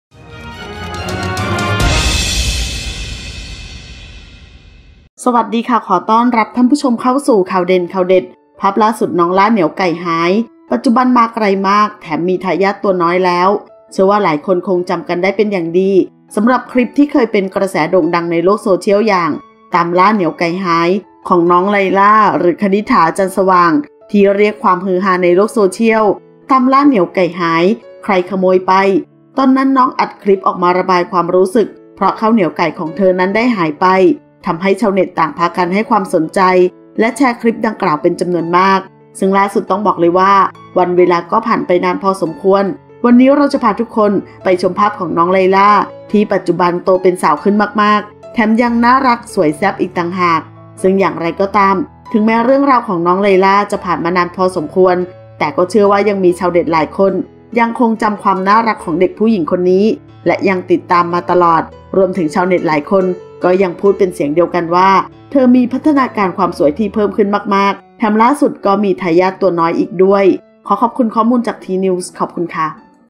ภาพล่าสุด น้องล่า เหนียวไก่หาย ปัจจุบันมาไกลมาก แถมมีทายาทตัวน้อยแล้ว
ขอบคุณที่มา :
ขอขอบคุณข้อมูลจาก: